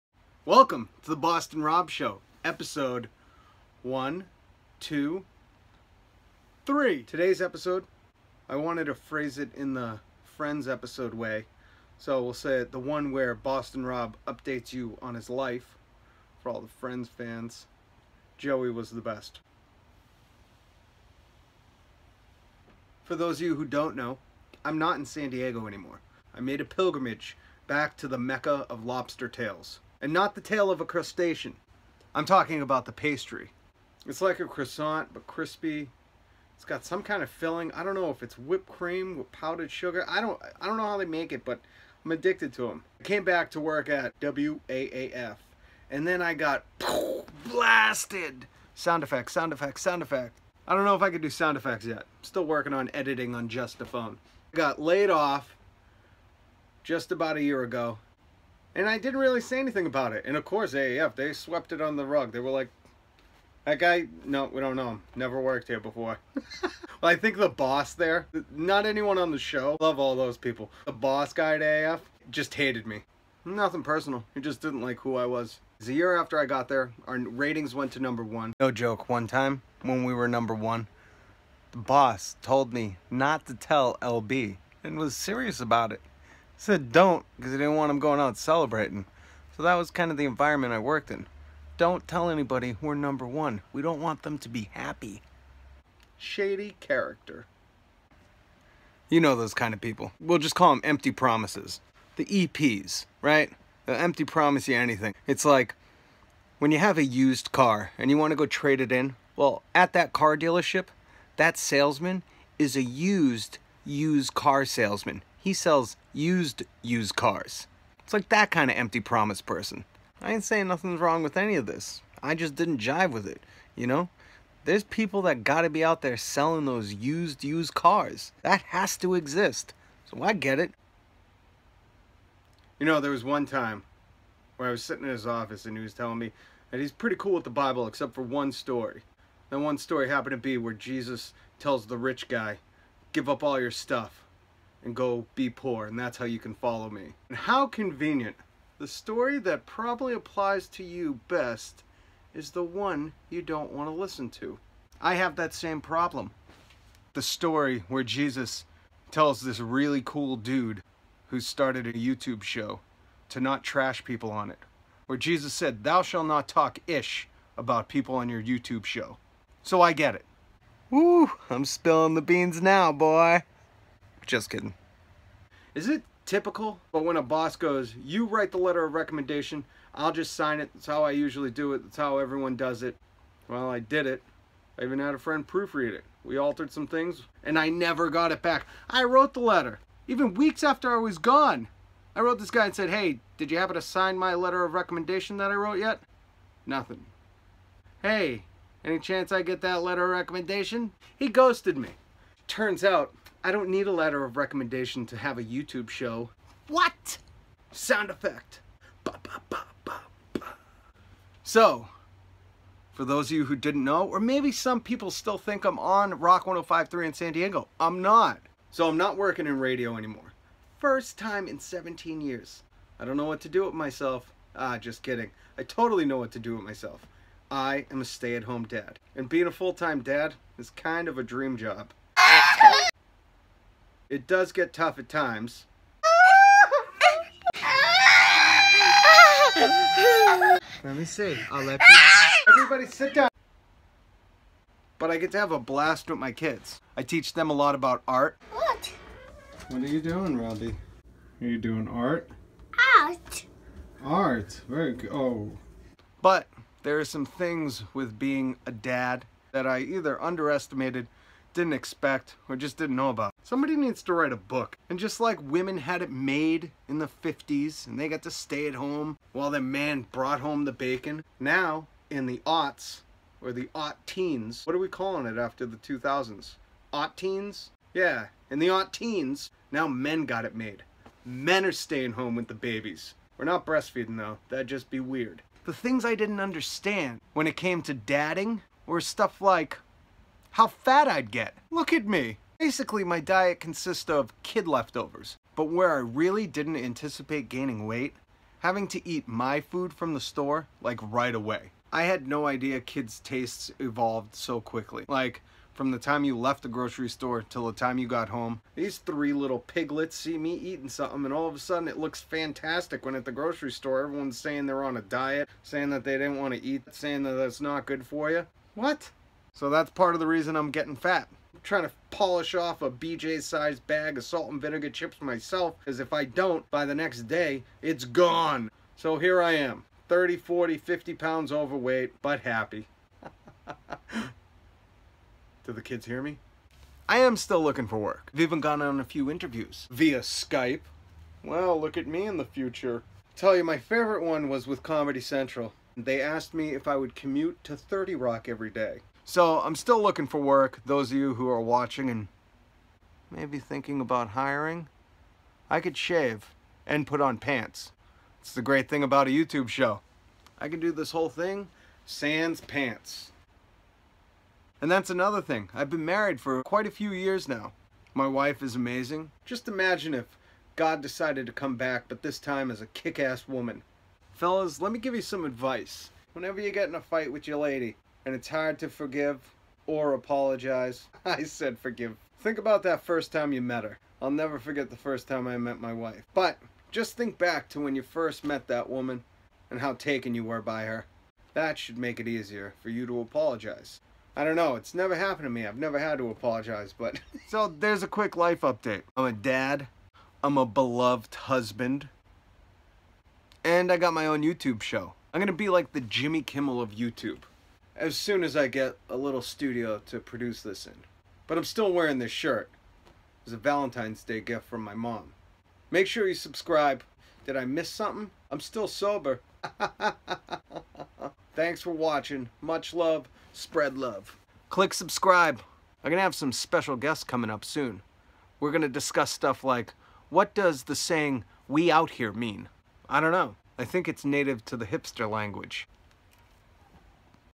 The Boston Robb Show - Episode Tré
The one where Robb gives an update on his life